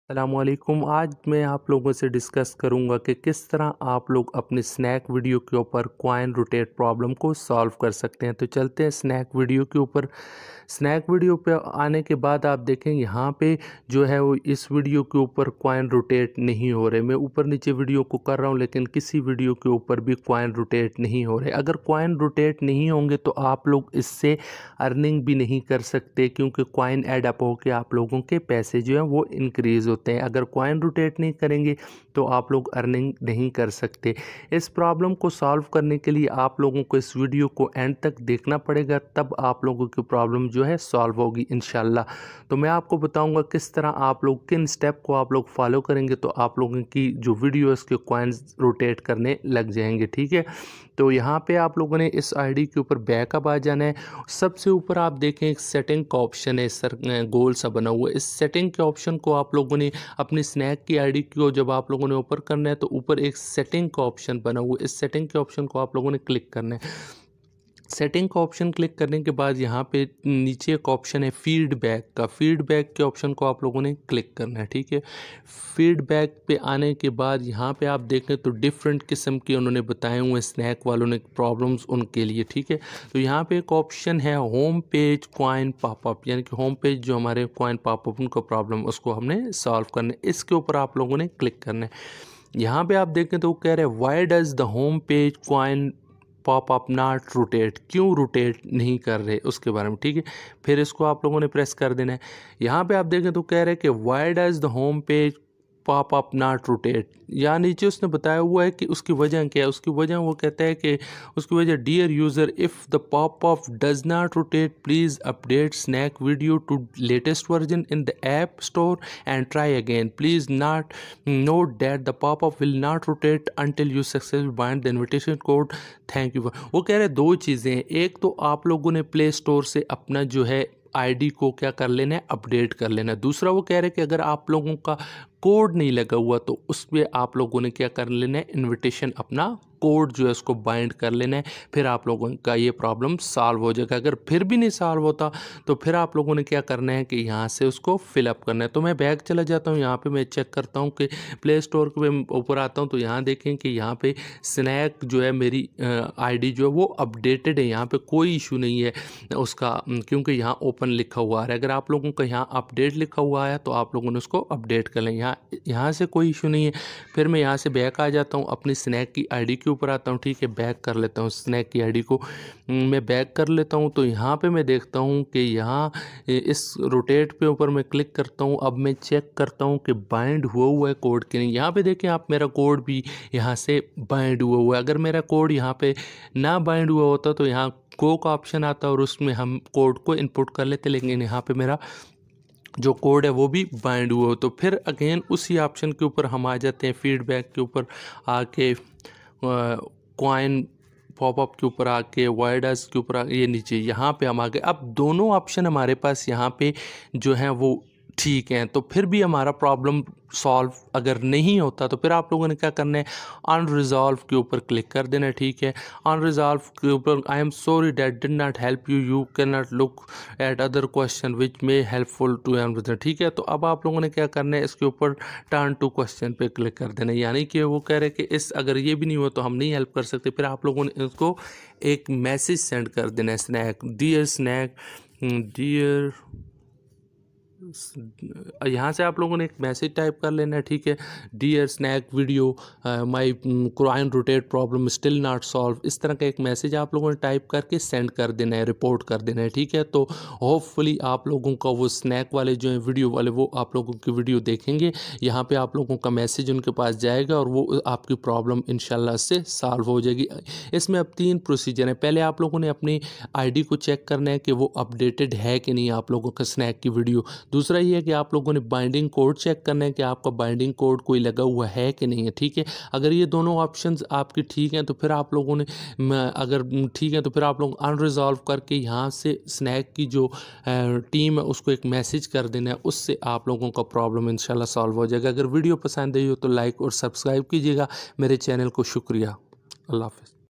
How to Solve Snack rotate problem/Coin rotate nahi kar rahe 100% solve provlem
Dosto agar ap ke snack video ma coin roster nahi kar raha to is video ma ma ap ko batao ga kis tarha ap is problem ko solve kar Sakta ha easily. My binding code is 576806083.